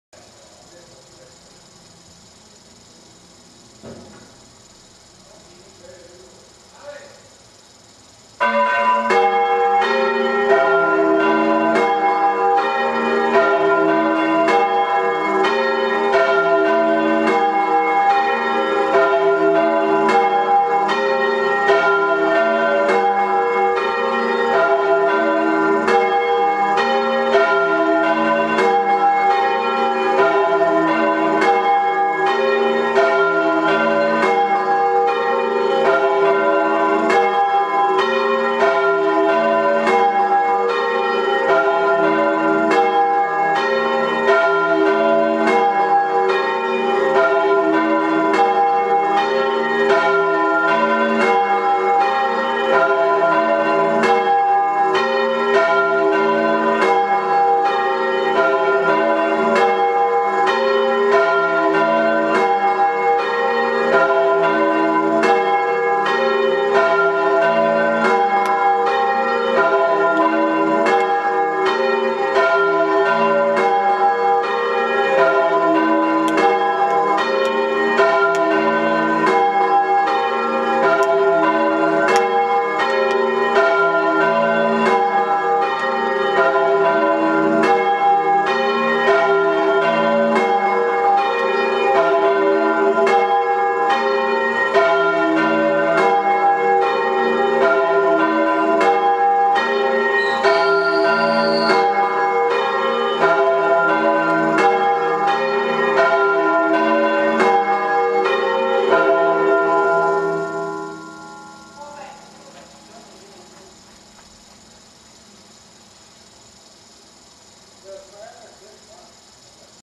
BOLOGNA.Certosa. Chiesa di S.Girolamo (05) v.060
Da un altra angolazione esecuzione di un pezzo in quarta(4-2-3-1).